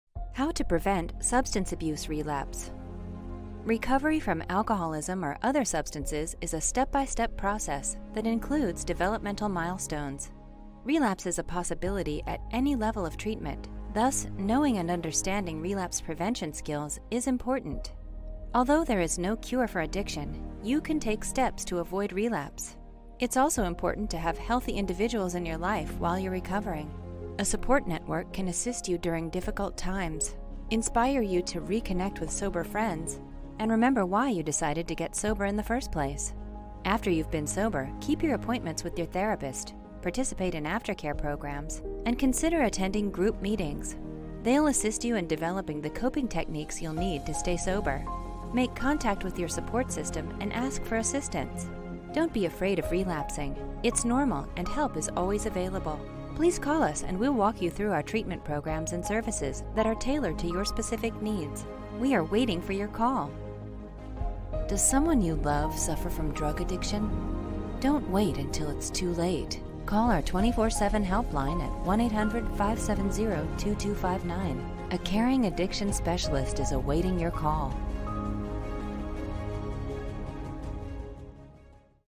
How To Prevent Substance Abuse Relapse?
Understanding common triggers maybe can help someone to avoid or minimize relapse in response to them. Relapse can be an indication that treatment needs to be reinstated or adjusted. Sticking with treatment for the entire length of the program is important, too.